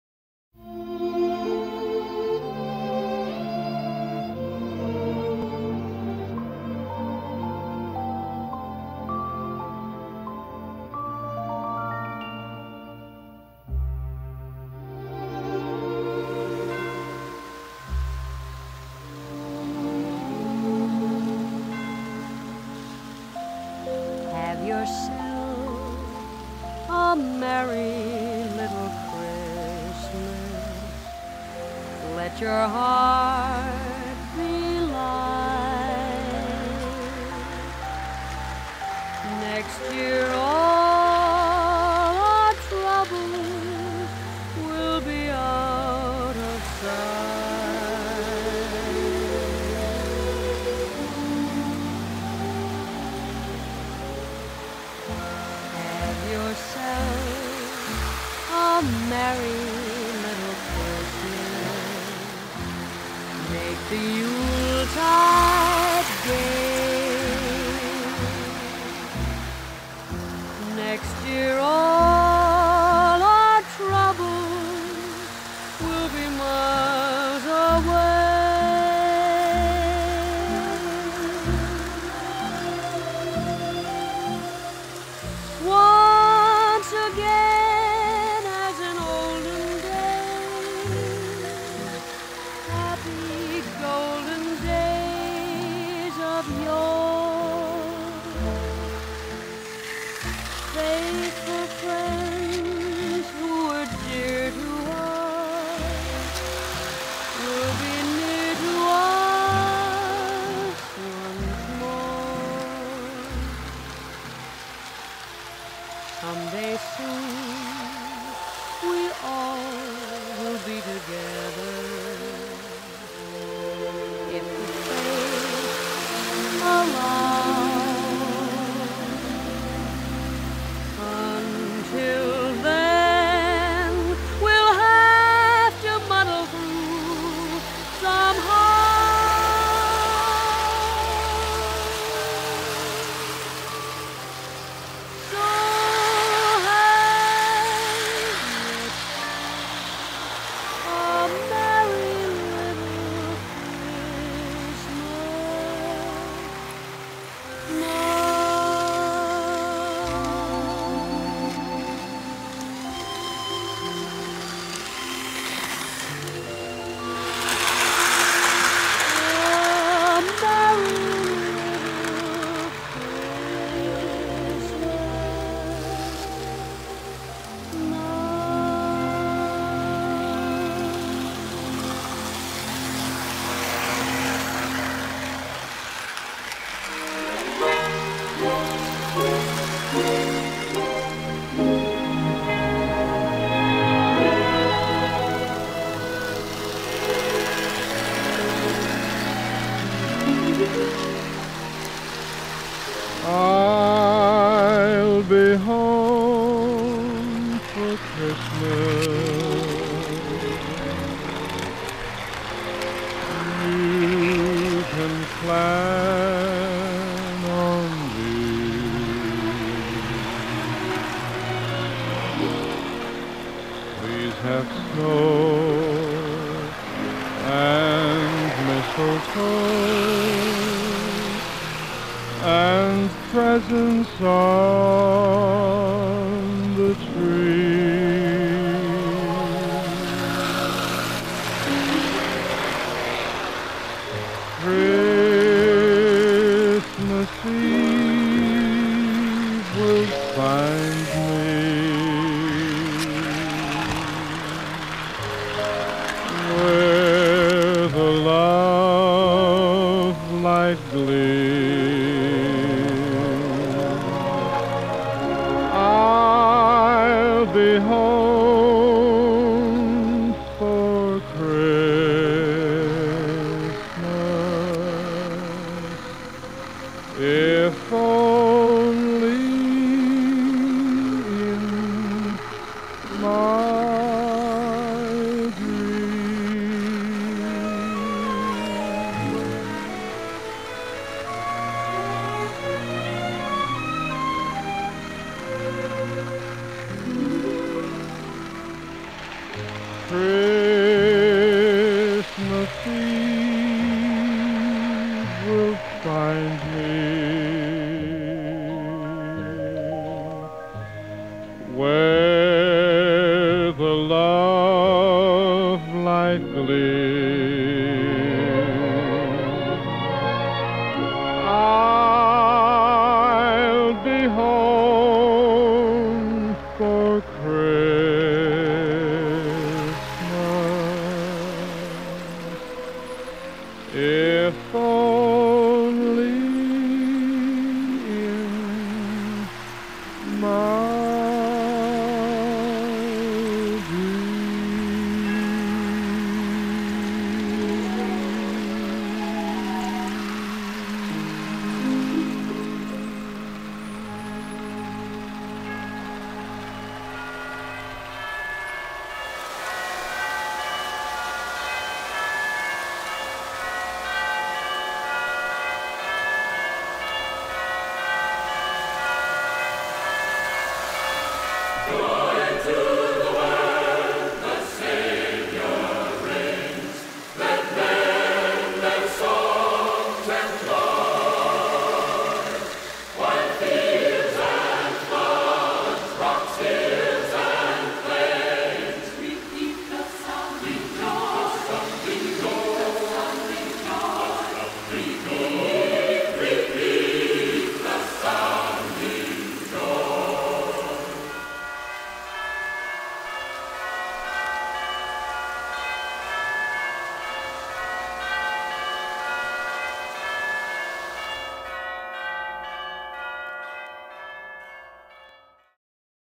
My Tree & Trains, 2017
My Christmas Tree, my Train layout & my Christmas village, "Neville", for 2017